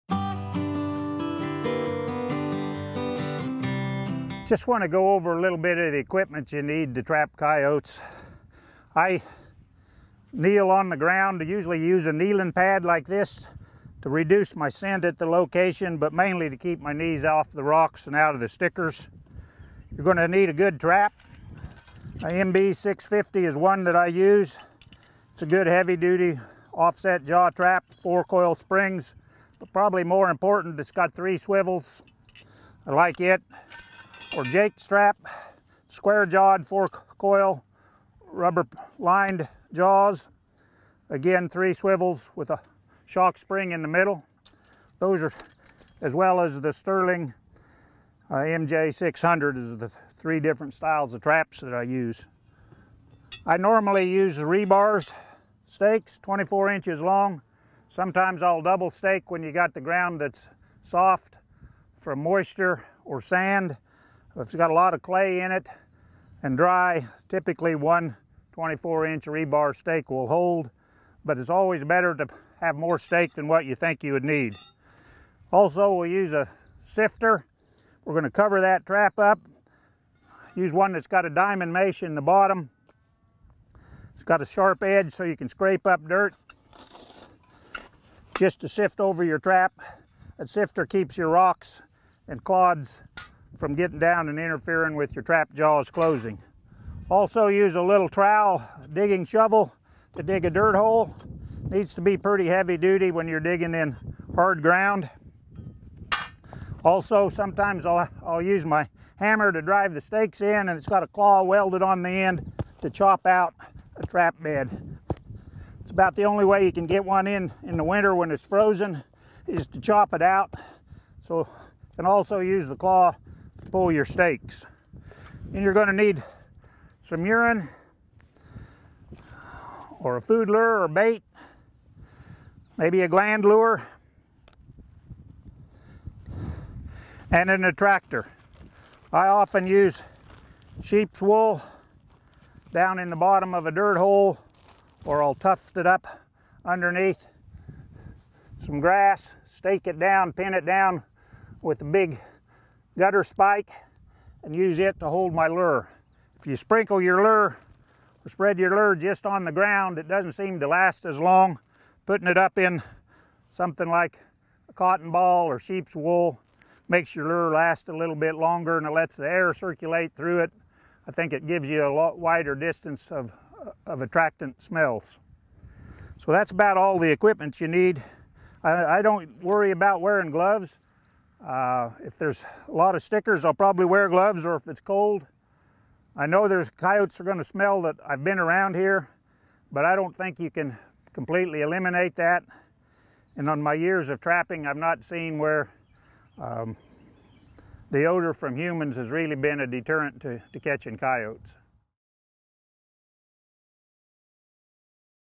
Coyote Trapping Equipment - Foothold
K-State Research and Extension Wildlife Specialist, Charlie Lee, explains the equipment needed to trap coyotes with foothold traps.